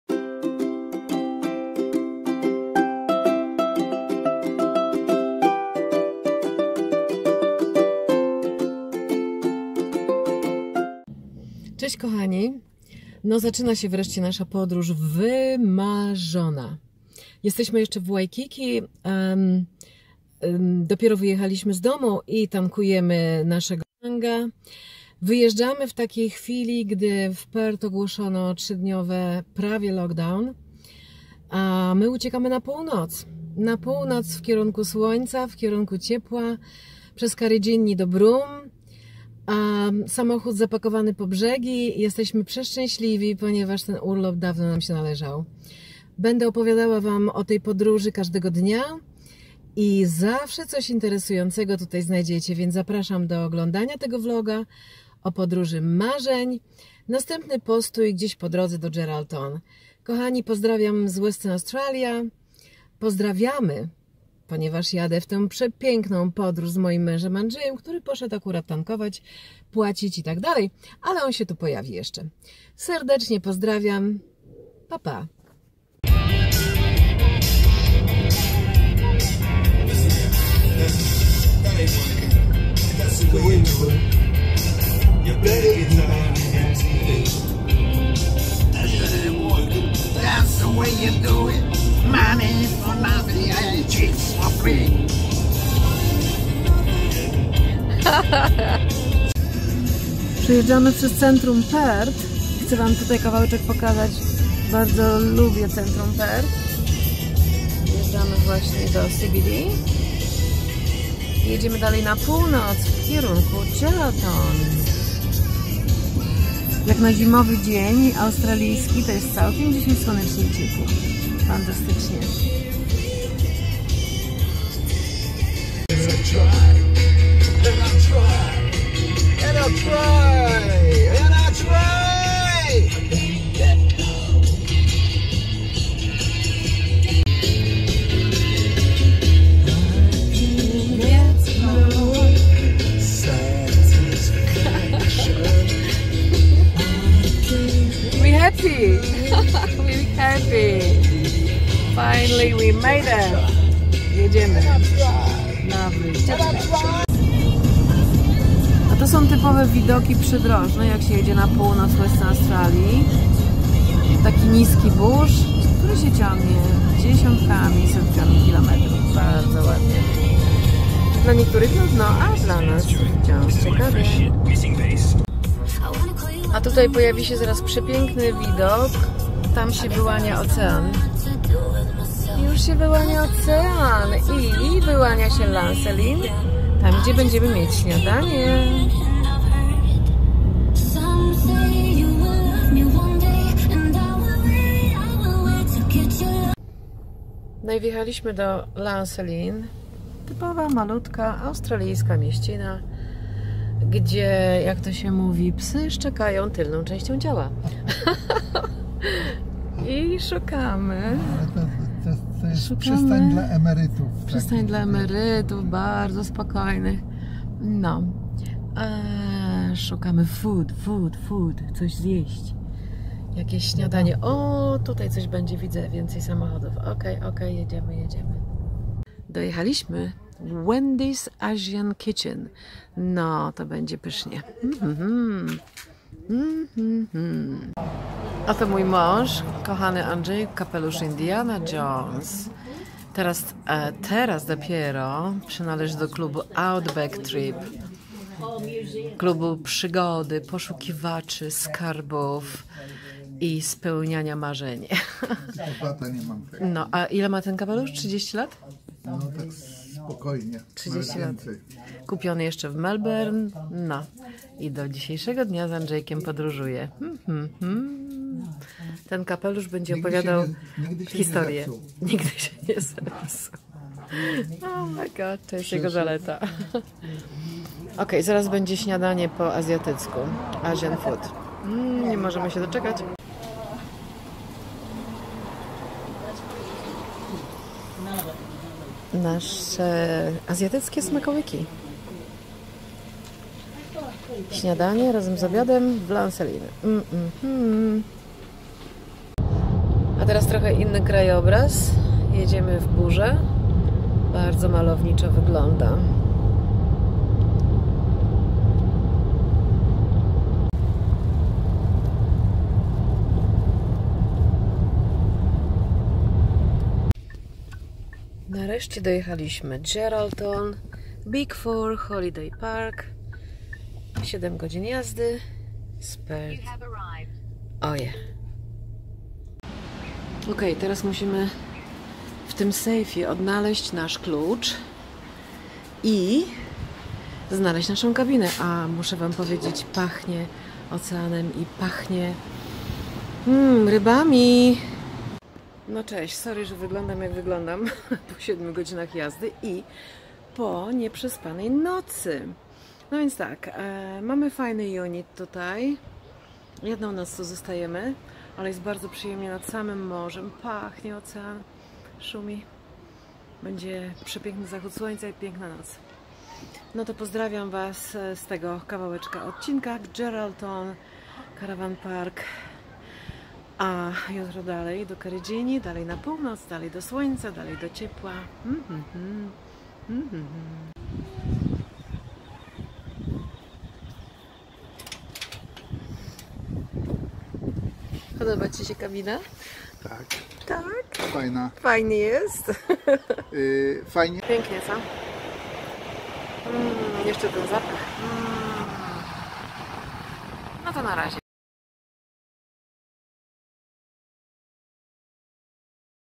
Geraldton. Z Perth do Karijinii. Australia zach. Vlog - Podróże Marzeń z Ewą May.
Wyjechaliśmy 28 czerwca, czyli w Australijską zimę, na północny wschód do Karijinii, najpiękniejszego i najstarszego zakątka czerwonej ziemi Australii. Pierwszy nocleg w Geraldton.